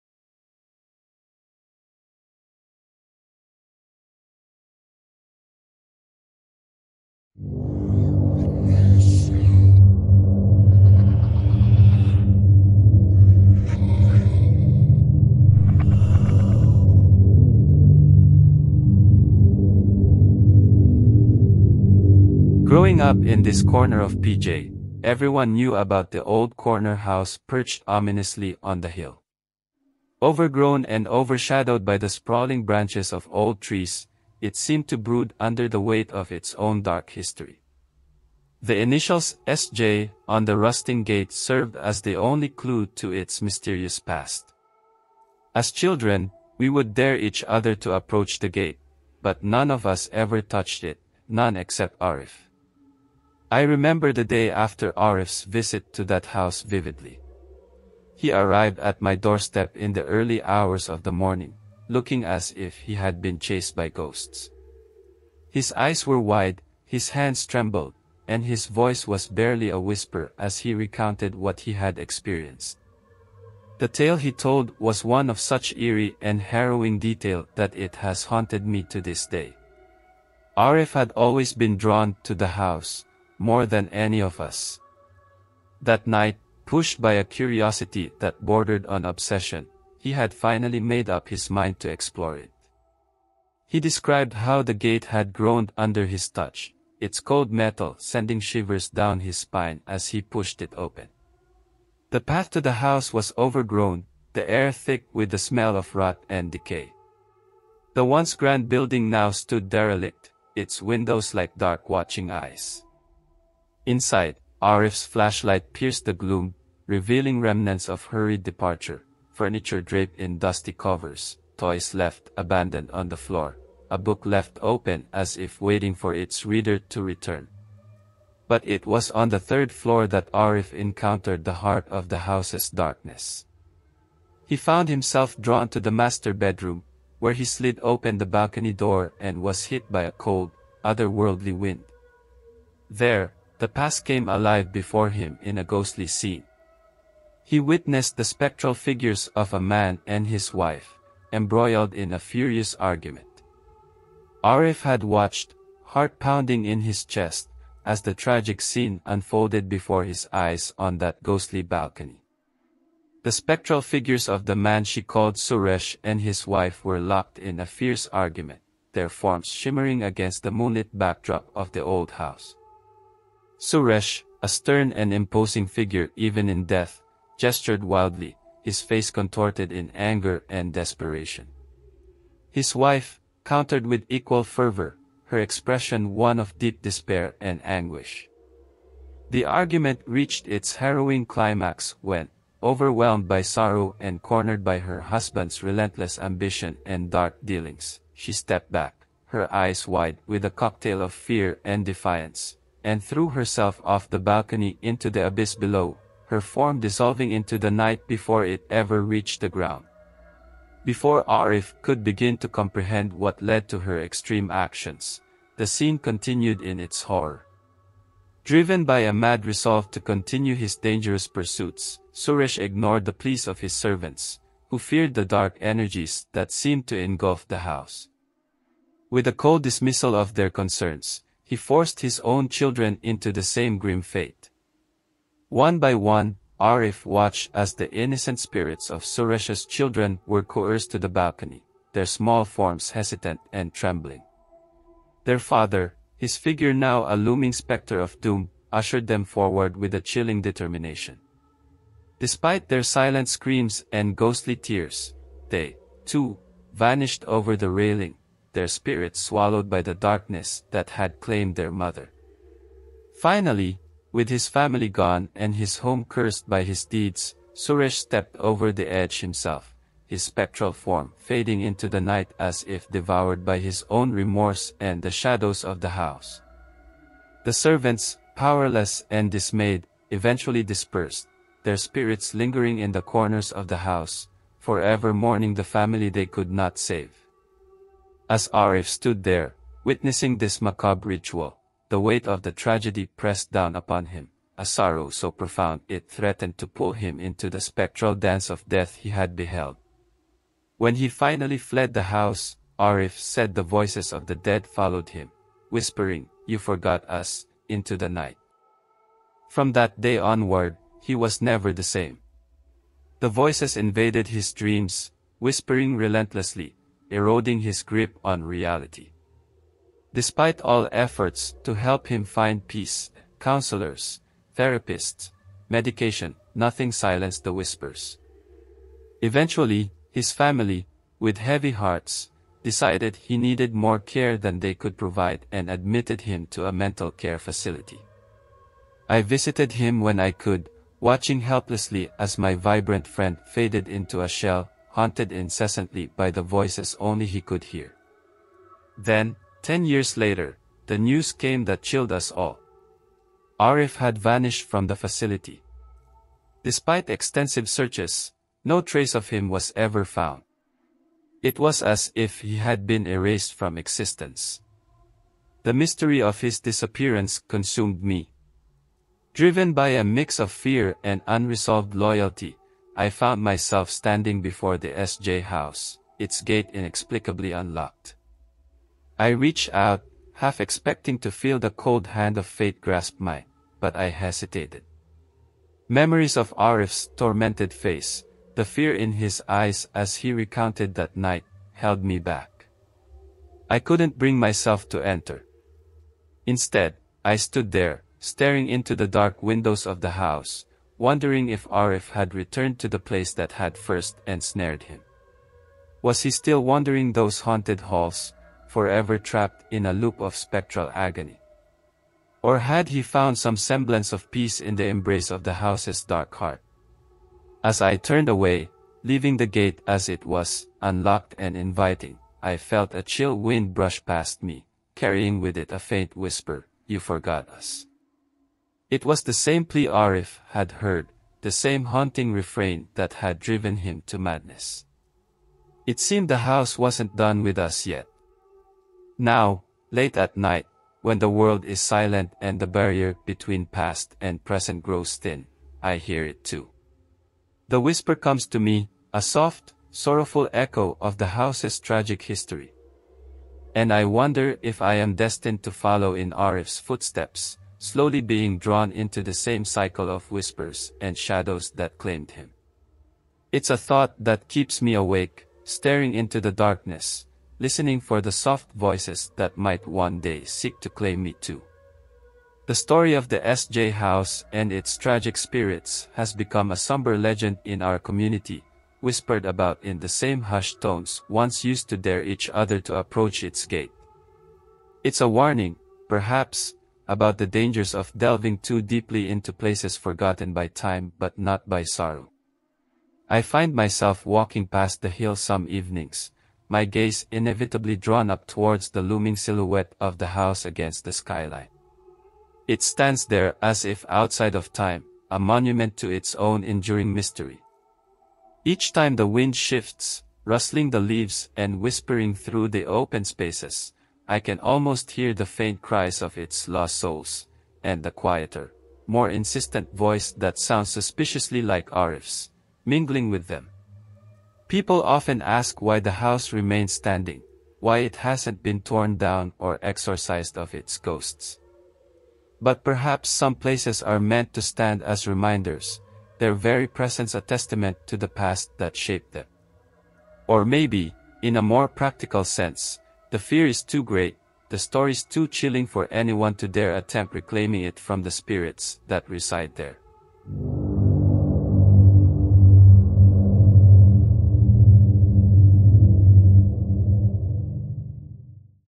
The ‘SJ’ House - A Horror Story by the BHC
It's time for another horror podcast . This one is a BHC original!
In this week's episode we listen to a chilling event that surrounds an old corner house on a Malaysian city hill, known only by the initials 'SJ.' This shadow-draped mansion, entwined in the overgrowth of time, harbors a dark history that has haunted the neighborhood for generations. Reading from the Killian Notebook, Marco Reyes unravels the horrifying tale of Arif, a young boy who dared enter the SJ house, forever altering his fate. What Arif witnessed inside those crumbling walls—a spectral tragedy replaying the last desperate hours of a doomed family—left him a shell of a man, haunted by whispers of the dead.
Tune in, if you dare, to confront the secrets of the SJ house and the ghostly lament that echoes through its halls. Exclusively on the Binturong Horror Channel.

Viewer and listener discretion is strongly advised.

Additionally, any reference to specific names, places, or locations is purely coincidental and not intended to correspond to any real individuals, events, practices, beliefs, places or locations.